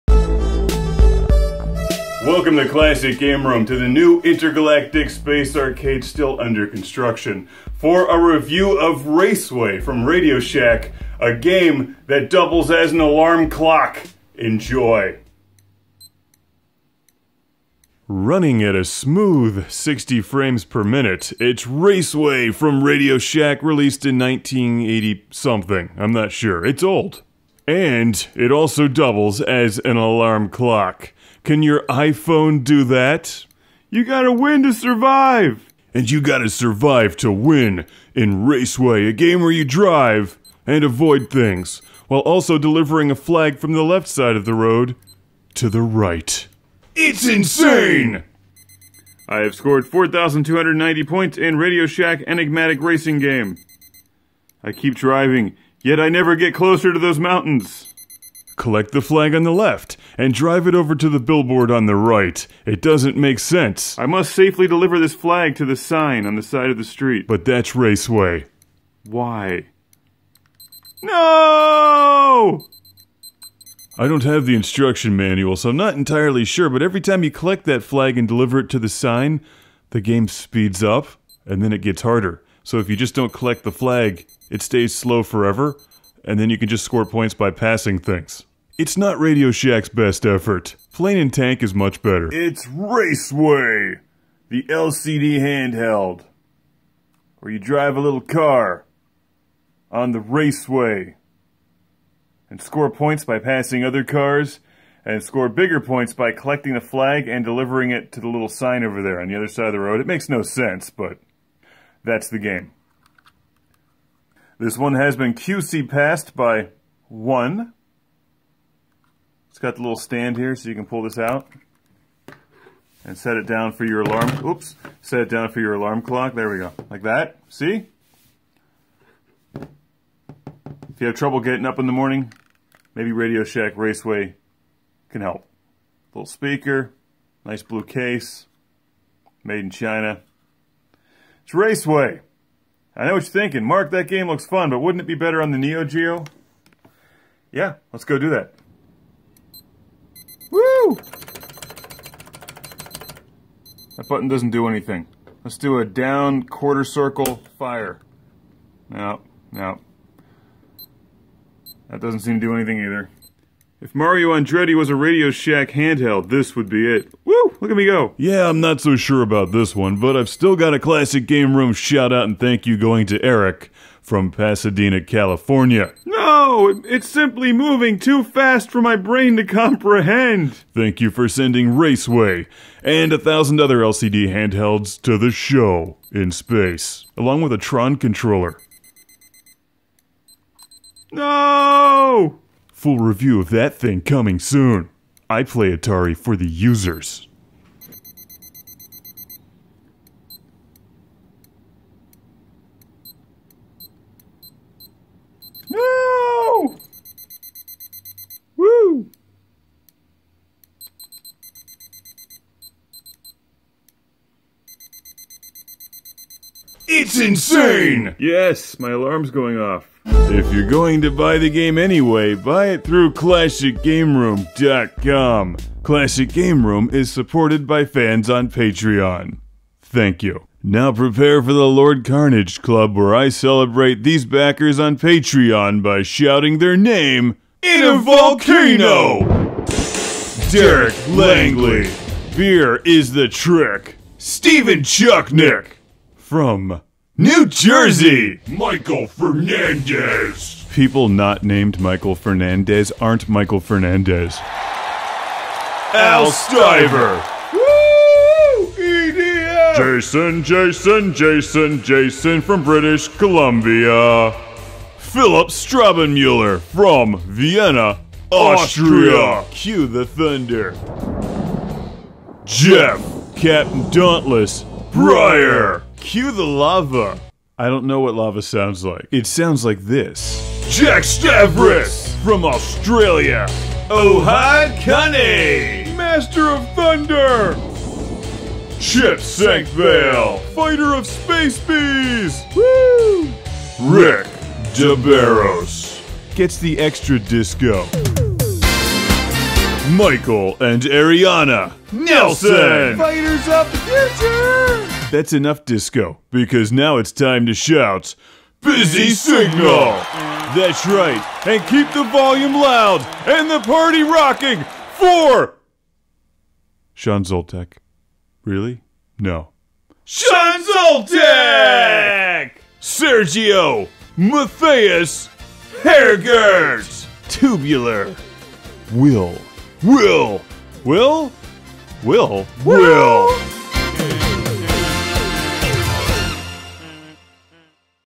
Classic Game Room - RADIO SHACK RACEWAY LCD handheld review
Classic Game Room reviews RACEWAY from Radio Shack, an old school LCD handheld video game that doubles as an ALARM CLOCK!! Beep beep beep. Time to play Radio Shack!!